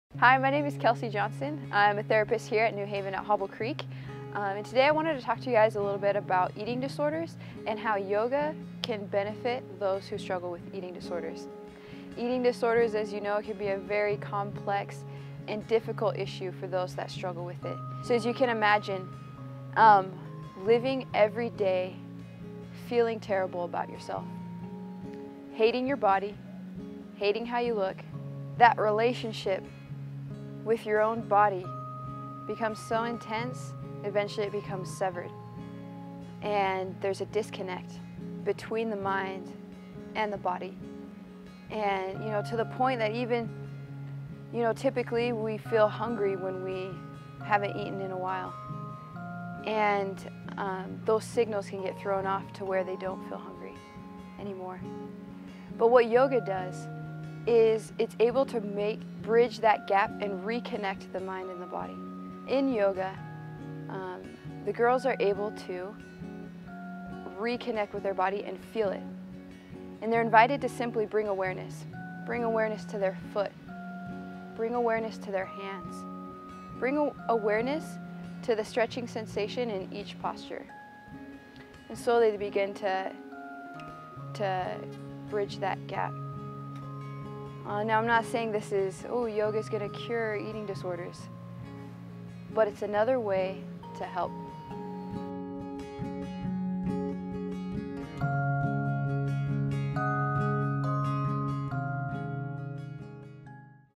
Treating Eating Disorders Through Yoga
Eating disorders create a disconnect between our minds and bodies.  In this video, Kelsey Johnson talks about how to use yoga to treat eating disorders and heal that disconnect.  Kelsey Johnson is a therapist at New Haven Residential Treatment Center.

for more information.